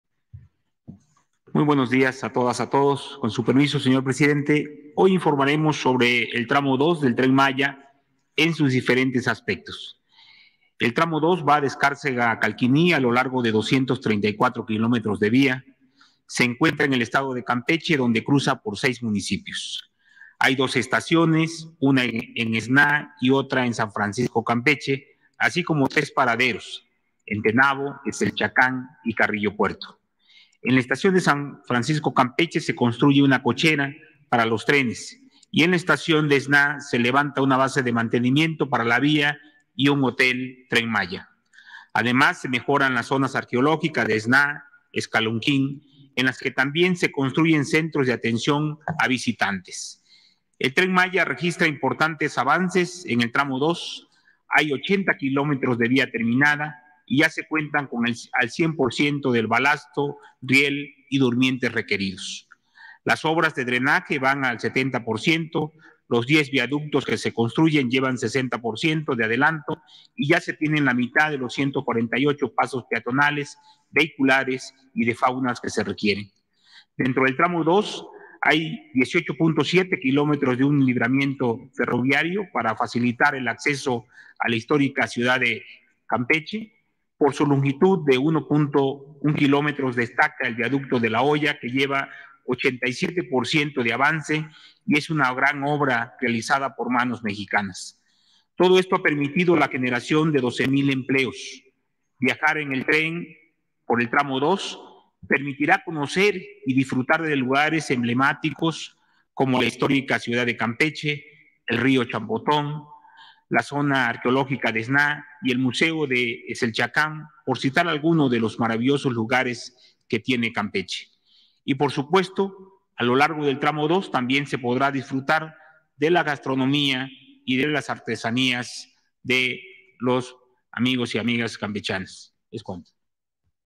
AVANZA EN FORMA EL TRAMO 2 DEL TREN MAYA CON 12 MIL EMPLEOS EN CAMPECHE
El director general del Fondo Nacional de Fomento al Turismo Javier May Rodríguez informó que el Tramo 2 del proyecto del Tren Maya que corre por Campeche tiene un avance con 80 kilómetros de vía ya terminada y se cuenta con el 100 por ciento de balastro requerido, además de que ya están listos la mitad de los 148 pasos peatonales, de vehículos y fauna, obras que han generado alrededor de 12 mil empleos y estará terminada en diciembre de 2023.